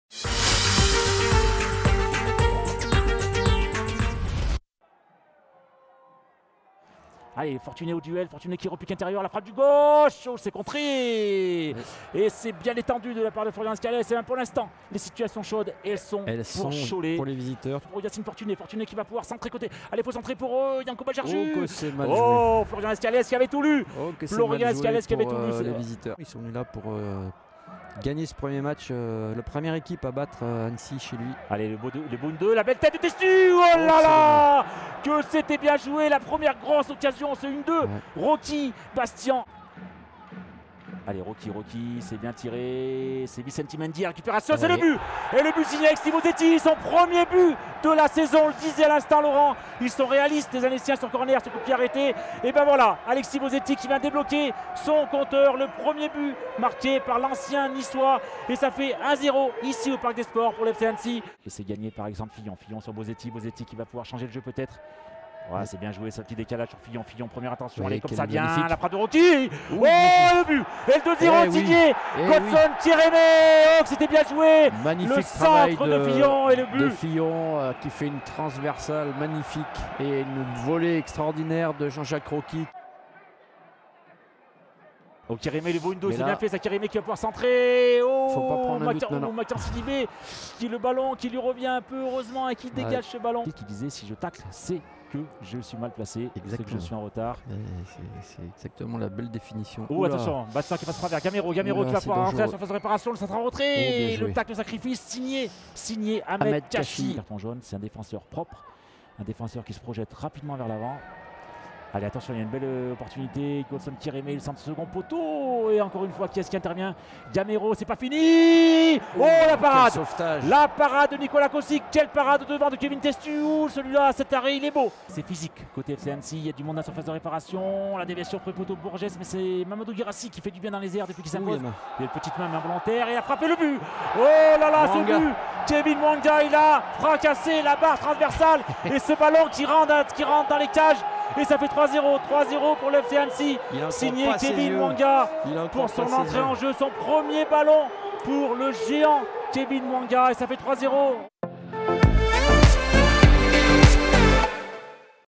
J11 I FC Annecy - SO Cholet (3-0), le résumé | National FFF 2021-2022
Retrouvez le résumé de la rencontre FC Annecy - SO Cholet  comptant pour la 11e journée du National (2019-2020).

Buts pour le FC Annecy : Alexy Bosetti (17e), Godson Kyeremeh (38e), Kevin Mouanga (86e)